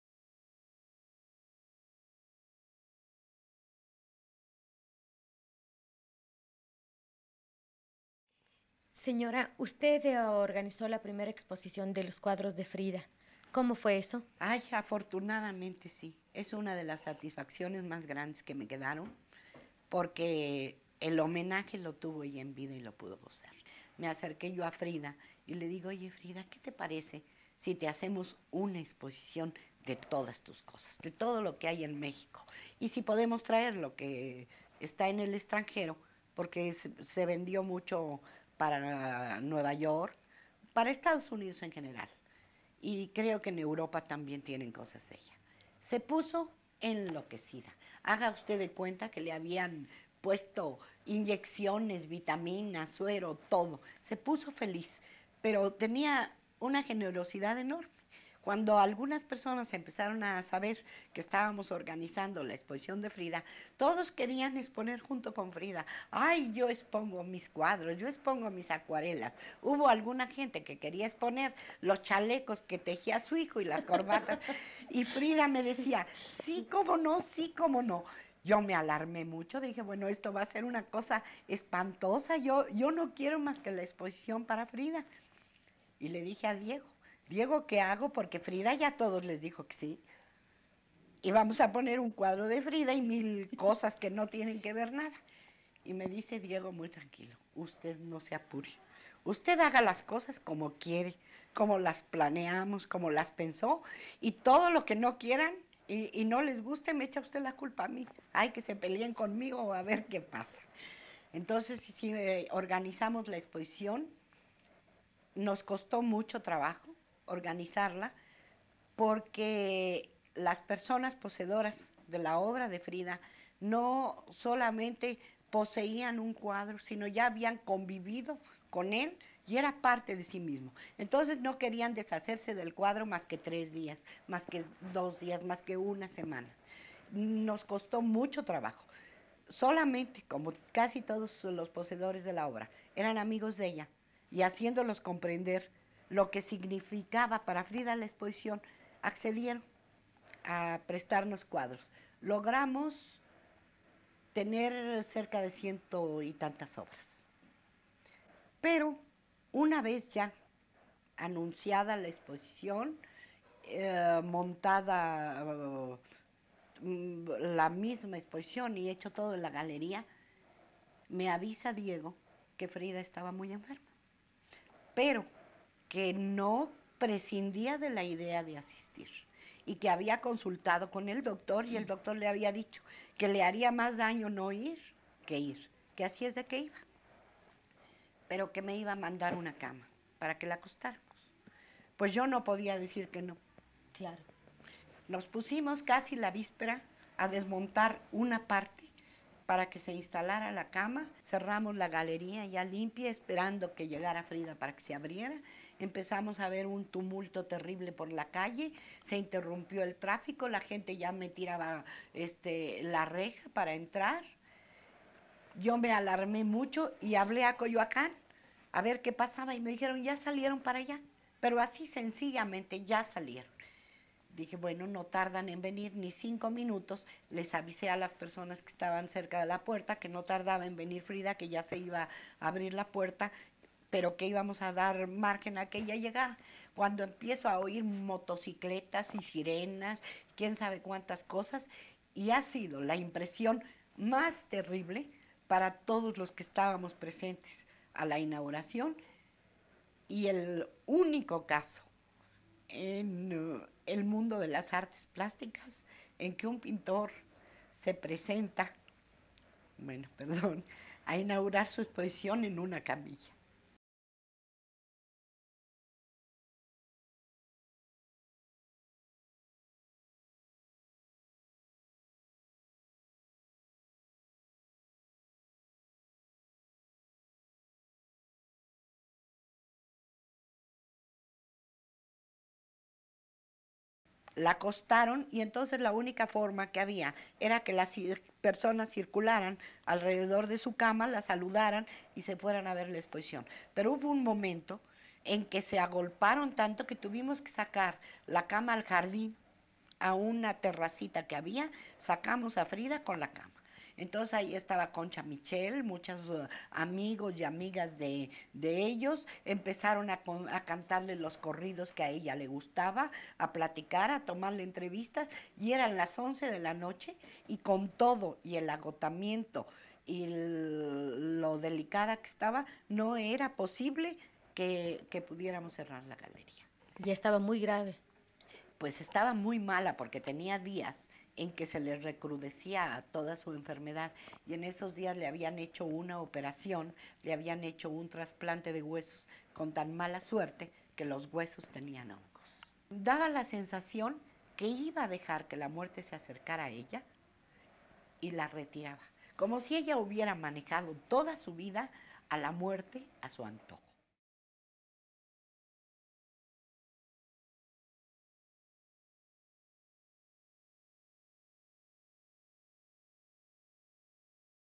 Lola Álvarez Bravo habla sobre Frida Kahlo
En esta entrevista, realizada alrededor de 1970, la fotógrafa Lola Álvarez Bravo da cuenta de la primera exposición de la pintora Frida Kahlo organizada por la propia Lola en la Galería de Arte Contemporáneo en México en 1953.